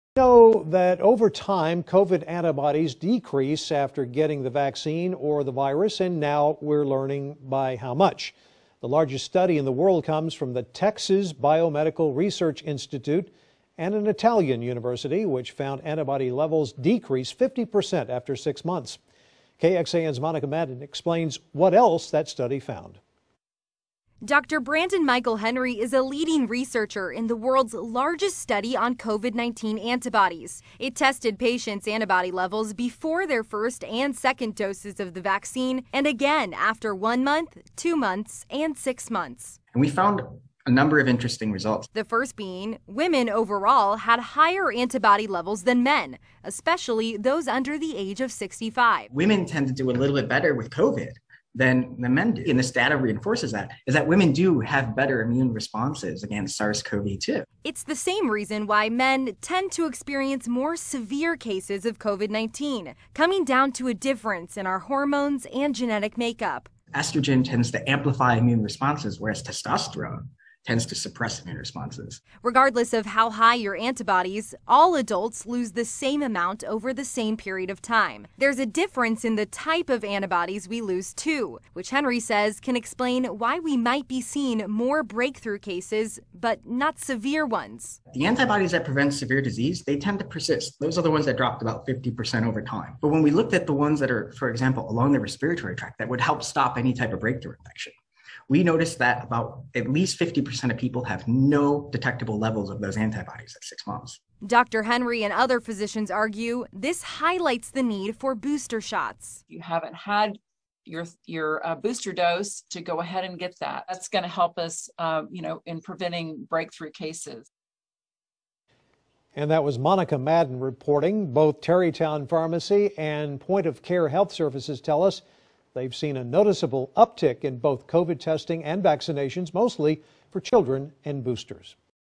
How Strong Are Your COVID-19 Antibodies?
Study shows it depends on age, and sex.